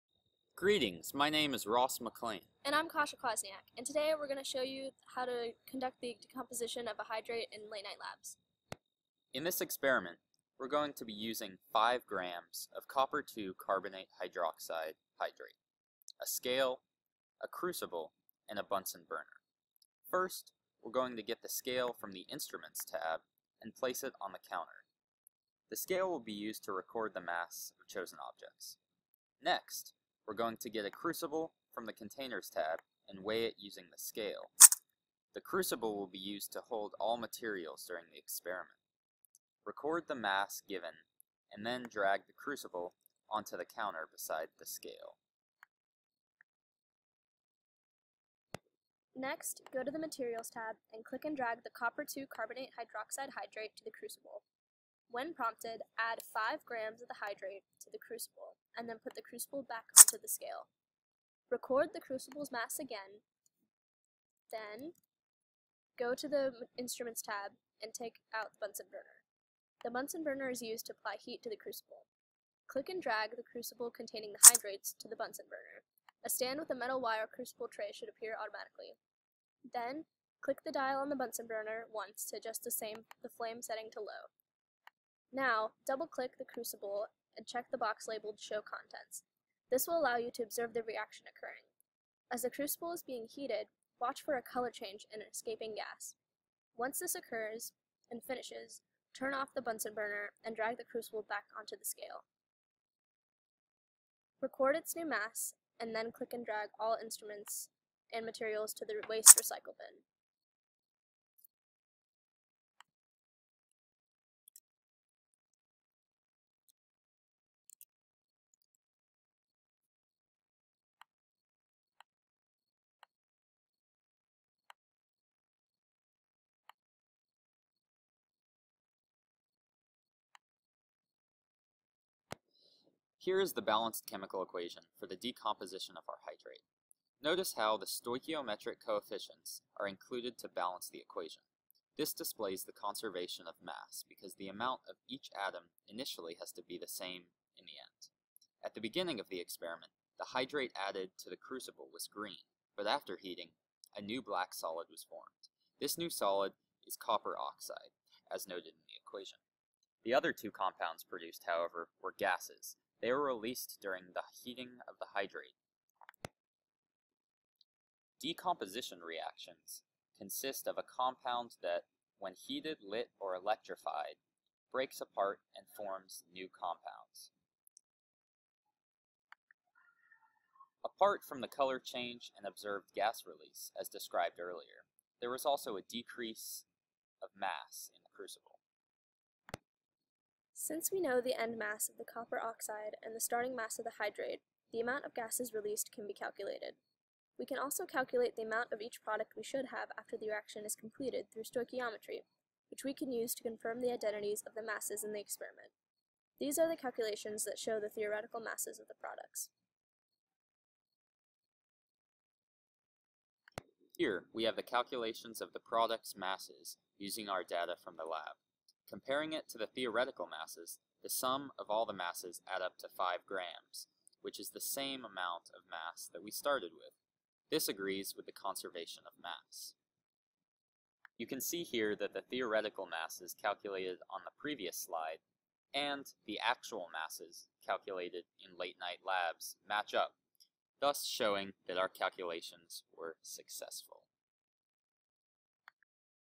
Decomposition of Copper(II) Carbonate Hydroxide Hydrate in LateNiteLabs
This video is about Chemistry